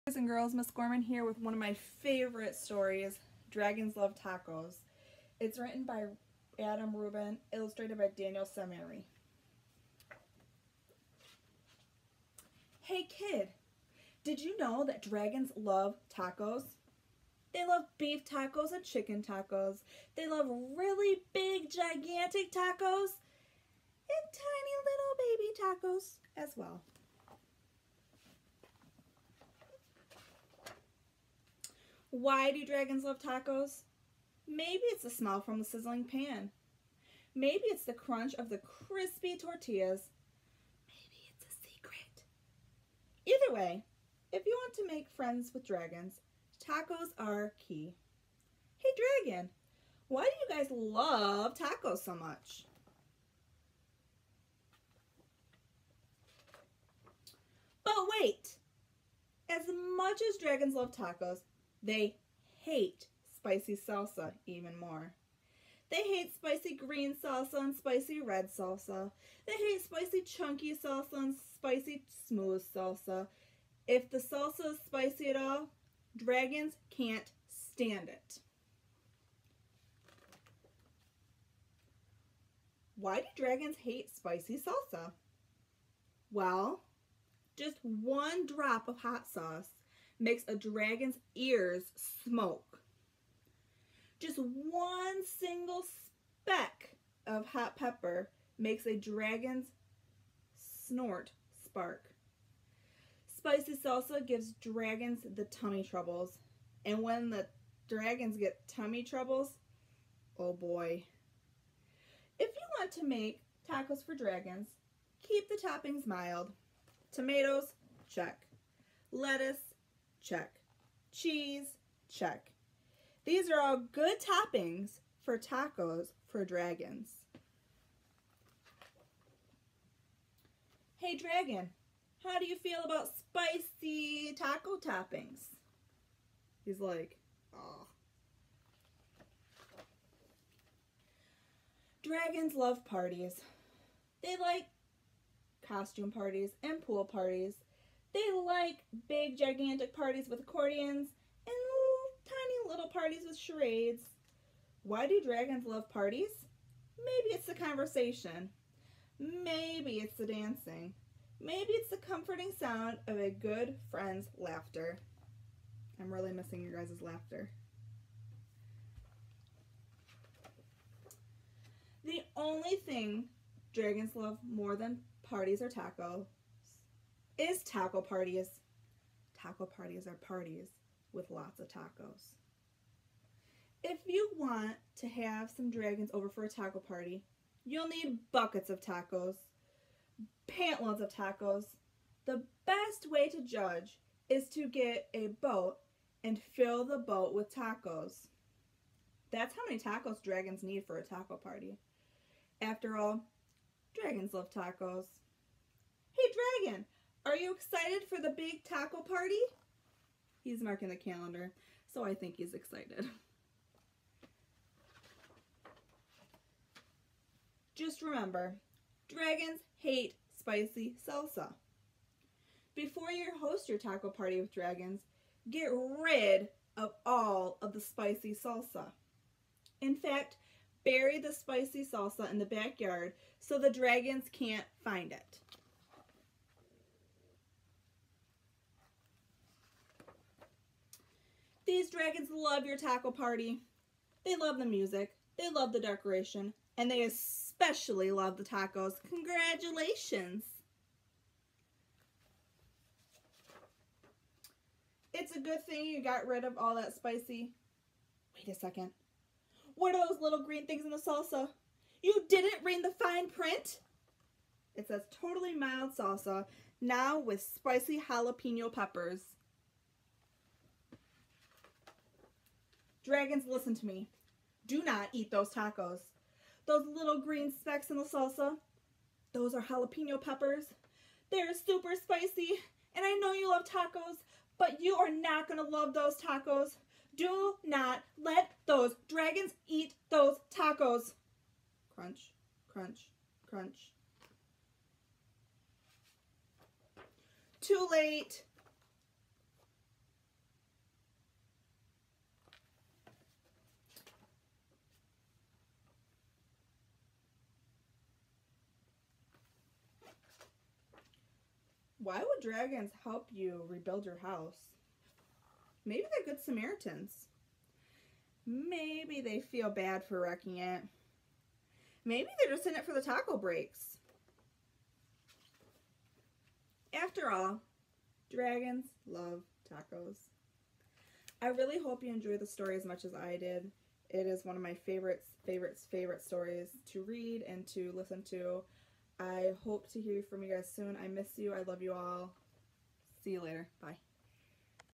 Dragons Love Tacos Read Aloud
I hope you enjoy this book! This is one of my favorites! :)

I do not claim ownership of this text.